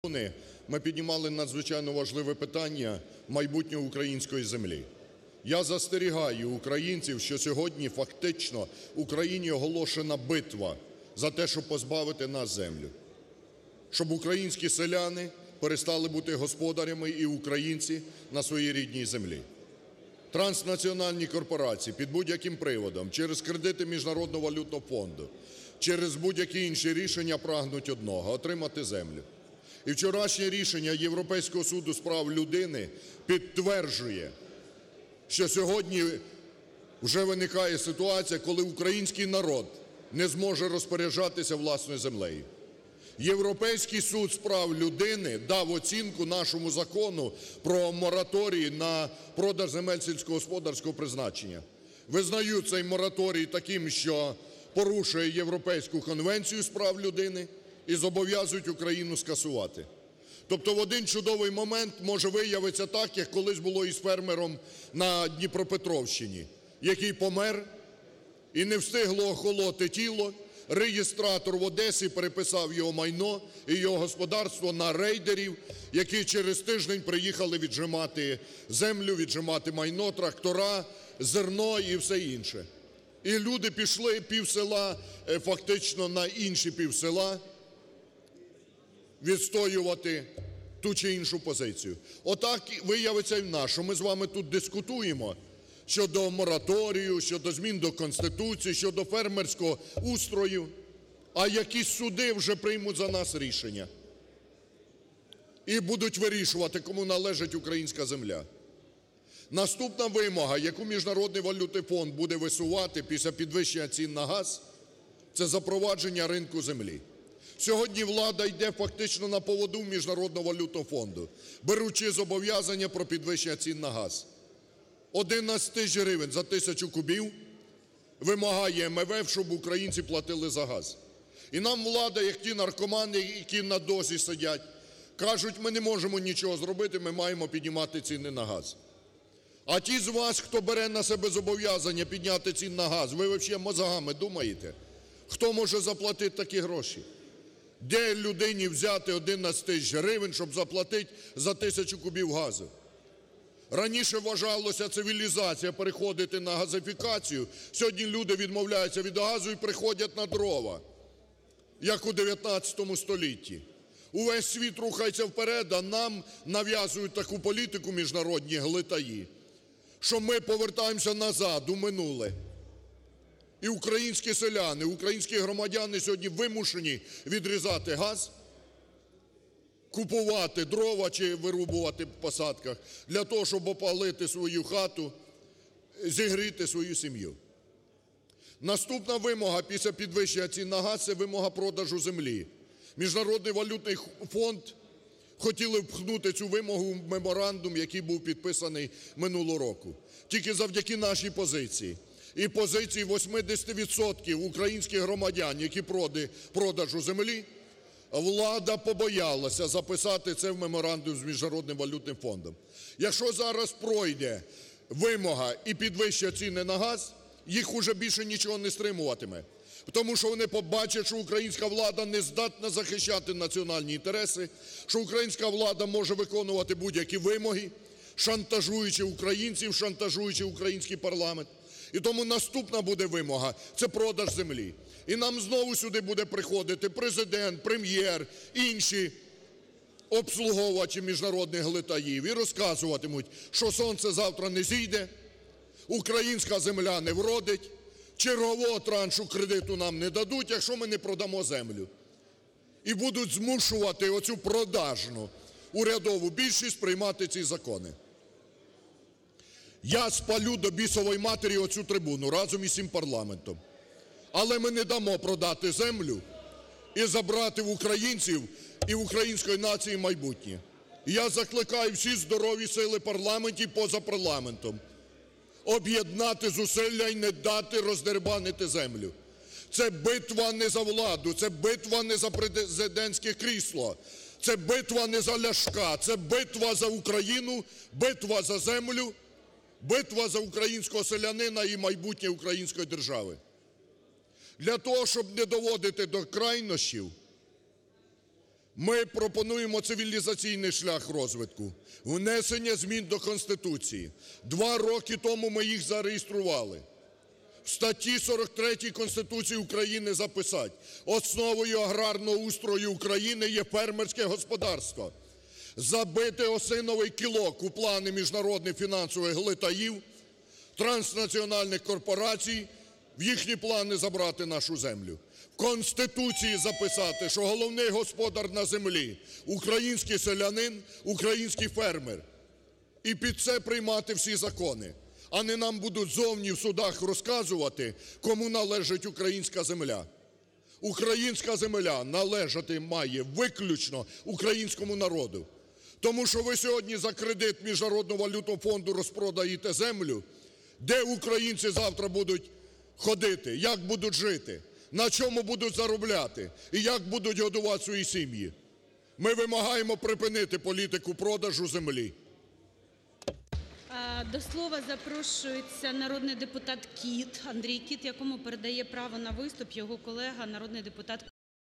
Ляшко: Спалю цю трибуну, але не дозволю позбавити українців землі!
В Україні оголошена битва за позбавлення українців землі. Її прагнуть отримати іноземні транснаціональні корпорації, шантажуючи кредитами МВФ. На цьому наголосив лідер Радикальної партії Олег Ляшко. Політик запевнив, що не дозволить продавати землю і зробити українців батраками.
«Європейський суд з прав людини дав оцінку нашому закону про мораторій на продаж земель сільськогосподарського призначення. Вони визнають цей мораторій таким, що порушує Європейську конвенцію з прав людини і зобов’язують Україну його скасувати. Тобто, в один чудовий момент може виявитися так, як із тим фермером на Дніпропетровщині – не встигло його тіло охолонути після смерті, а реєстратор з Одеси переписав його майно і господарство на рейдерів, які через тиждень приїхали віджимати землю, трактори, зерно і все інше. Таке може статися і в Україні: поки ми тут дискутуємо щодо мораторію, якісь суди вже приймуть за нас рішення і українці землею вже не розпоряджатимуться», - підкреслив Ляшко.
Лідер РПЛ переконаний, погоджуватися на умови МВФ у збиток українцям не можна.
«Нинішня влада йде на поводу МВФ, який хоче, щоб українці платили 11 тисяч гривень за тисячу кубів газу. Влада, як ті наркомани, які сидять на дозі. Каже, що не може нічого зробити, бо не отримаємо черговий транш. Ті, хто беруть ці зобов’язання, думають мізками? Хто може заплатити такі гроші за газ? Раніше вважалося розвитком цивілізації переходити на газ, а сьогодні українці відмовляються від газу і переходять на дрова. Як у ХІХ столітті. Весь світ рухається вперед, а нам міжнародні глитаї нав’язують таку політику, з якою ми повертаємося назад у минуле. І українці будуть змушені відрізати газ, купувати дрова чи вирубувати їх в посадках, щоб обігріти свою хату і зігріти свою сім’ю!», - обурився народний депутат.
Олег Ляшко жорстко розкритикував українську владу за слабкодухість, коли мова йде про надзвичайно важливі для українців питання.
Тільки завдяки нашій позиції і позиції 80% українців влада побоялася записати це в меморандум. Якщо зараз пройде вимога про підвищення цін на газ, МВФ більше нічого не стримуватиме. Вони побачать, що українська влада не здатна захищати національні інтереси і може виконувати будь-які вимоги, шантажуючи українців і український парламент. І сюди знову приходитимуть Президент, Прем’єр-міністр та інші обслуговувачі міжнародних глитаїв і розказуватимуть, що сонце завтра не зійде чи українська земля не вродить, якщо ми не будемо продавати землю. Я спалю до бісової матері цю трибуну і парламент, але не дам продати землю і забрати в української нації майбутнє!», - наголосив лідер РПЛ.
Олег Ляшко закликав всі політичні сили об’єднати зусилля і не дати продавати українську землю.
«Це битва не за владу чи президентське крісло. Це битва за Україну, українського селянина і майбутнє української держави. Ми пропонуємо цивілізаційний шлях вирішення цієї проблеми – внести зміни до Конституції, що основою аграрного устрою країни є фермерські господарства, і забити осиновий кілок у плани міжнародних глитаїв та транснаціональних корпорацій забрати українську землю. Головний господар української землі – український селянин і український фермер. Українська земля має належати виключно українському народу», - підсумував політик.